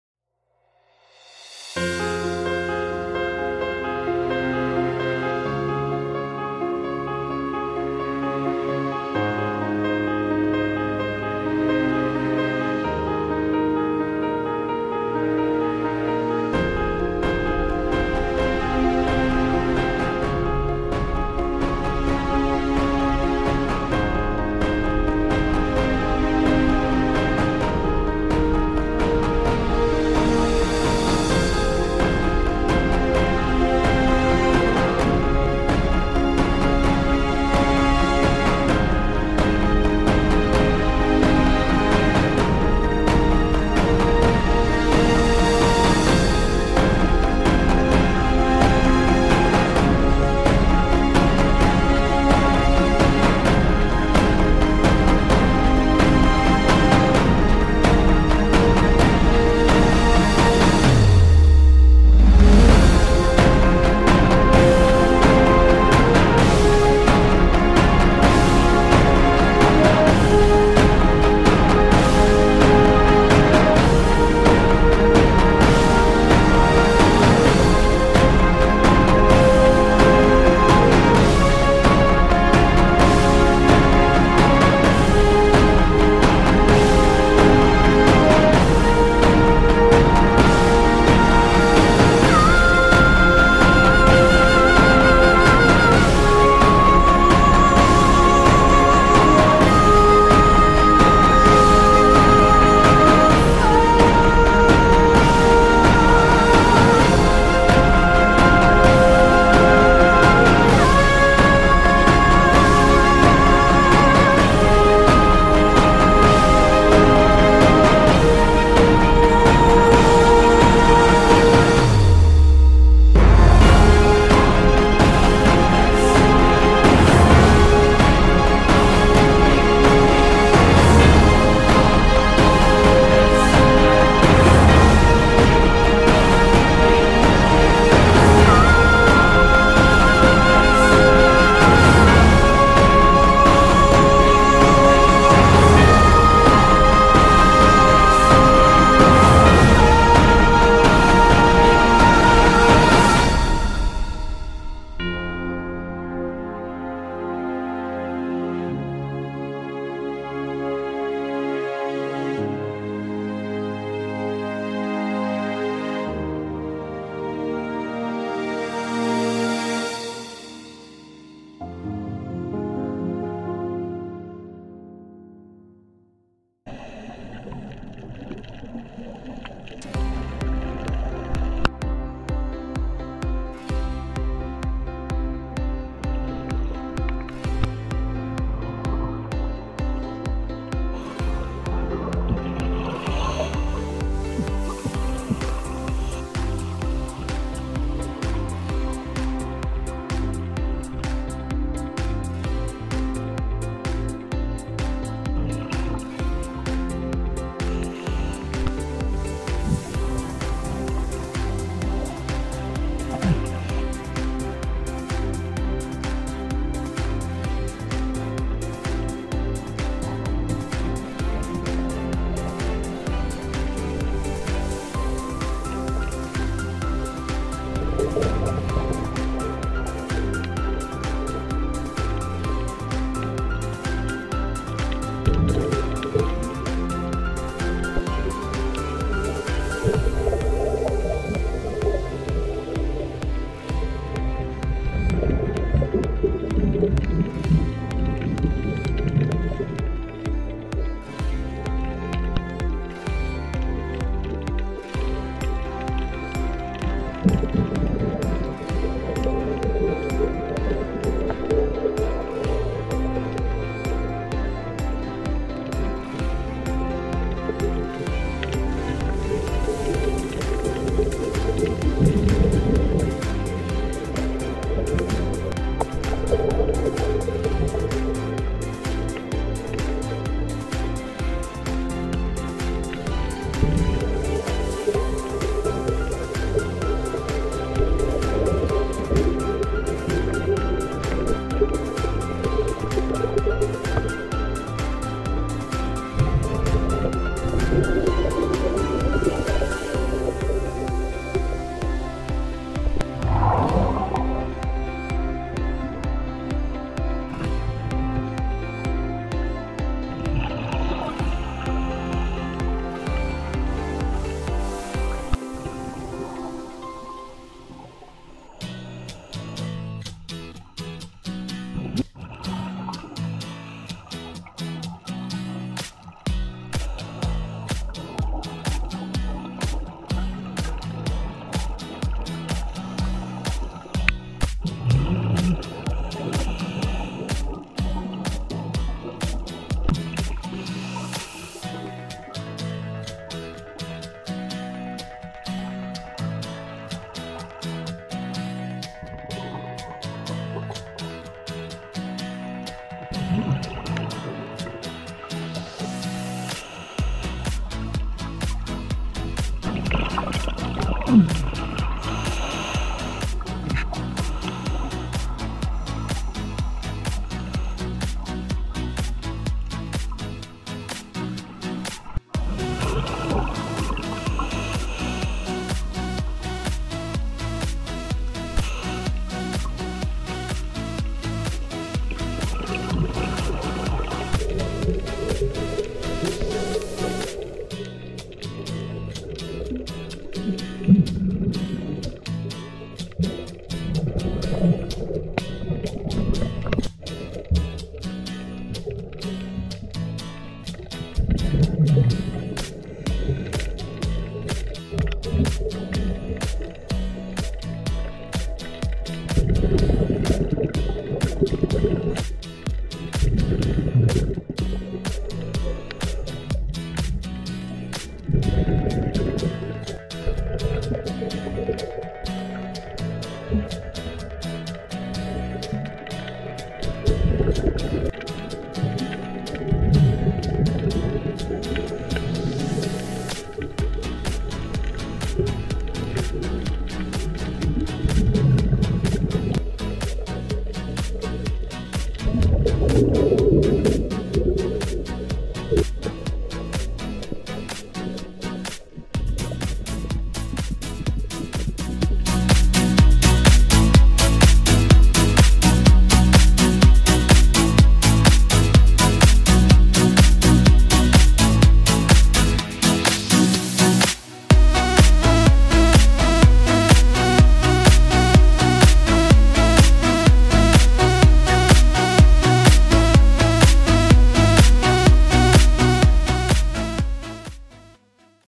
Tauchen an der Insel Brač 4 K-Video (29.05.2024)
Tieftauchgang an der Insel Brač in Kroatien